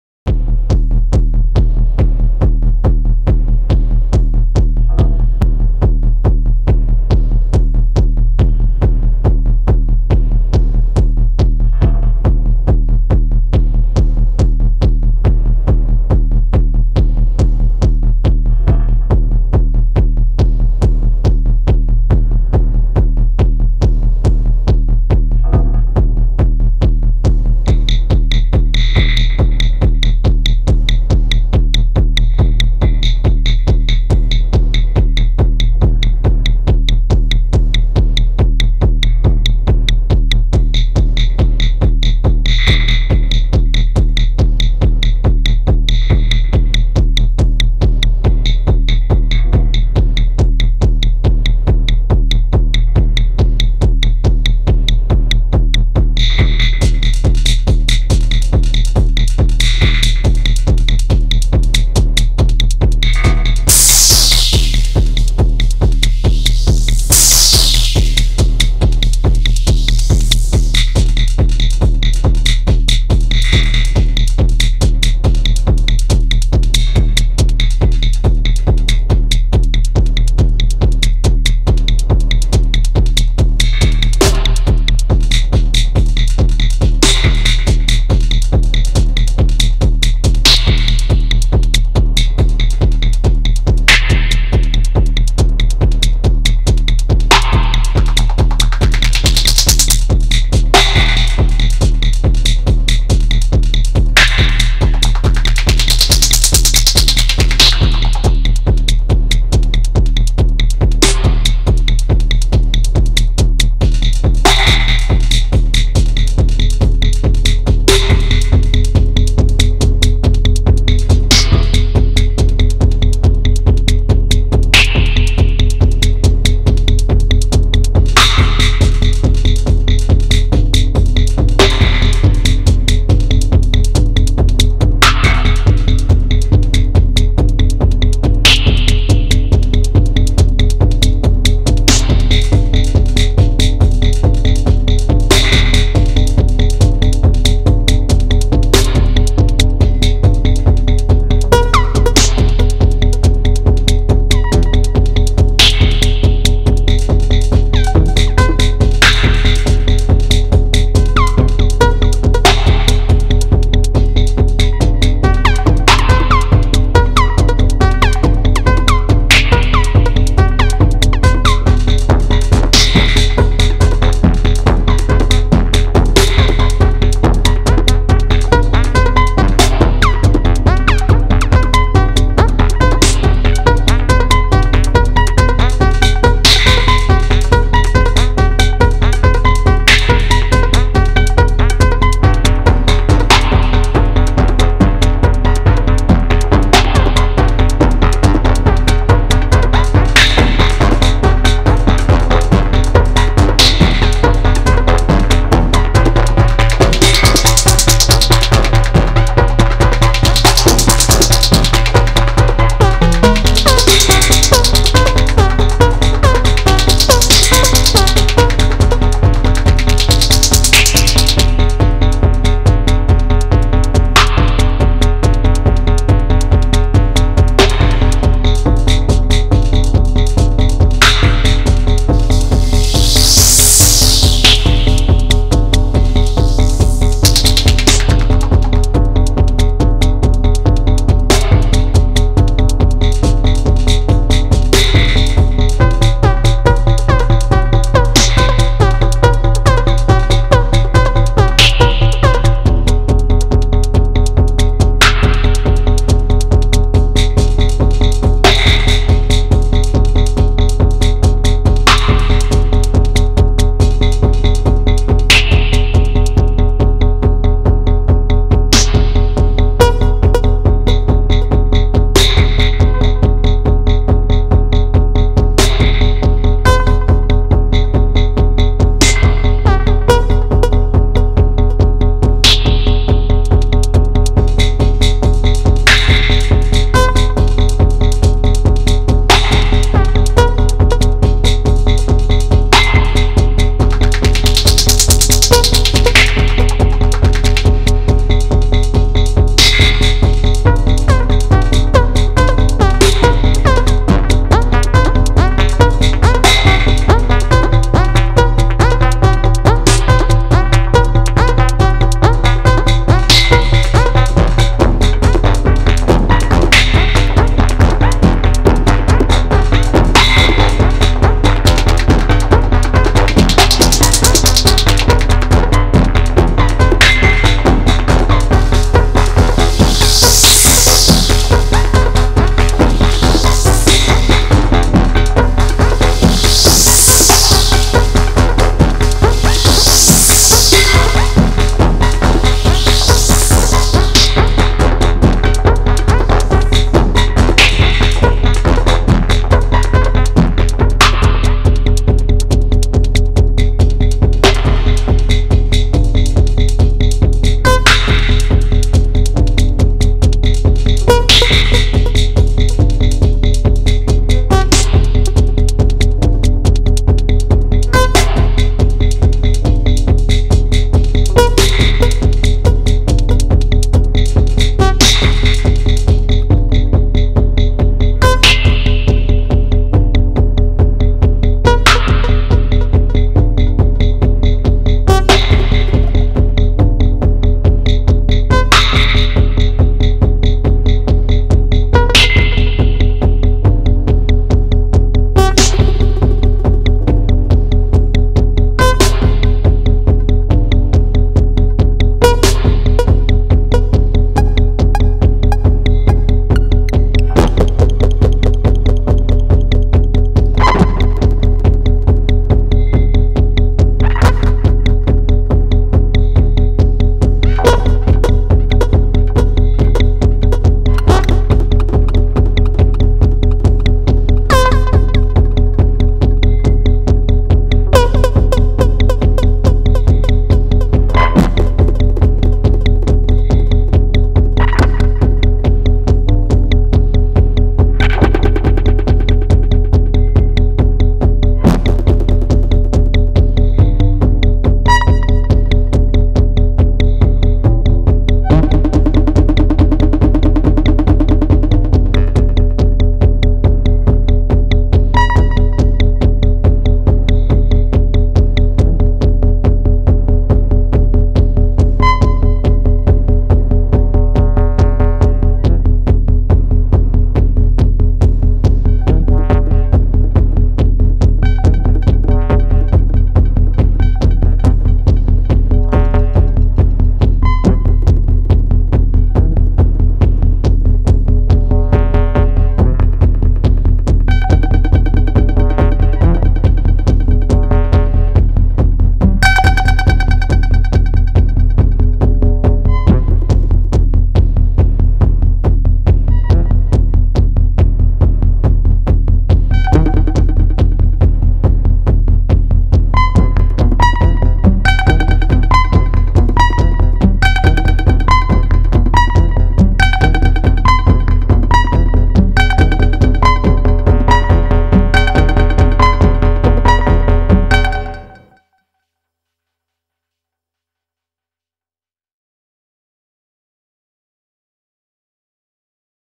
treno (eurorack minimal techno)
voices in this patch:

kick, snare, hats, pcsn by Expert-Sleepers Disting Ex (SD6 Triggers algo.)
low synth by Instruo Ts-L (sinus wave)
high synth by Mannequins Mangrove

images captured en route Milano - Venezia on board the hi-speed train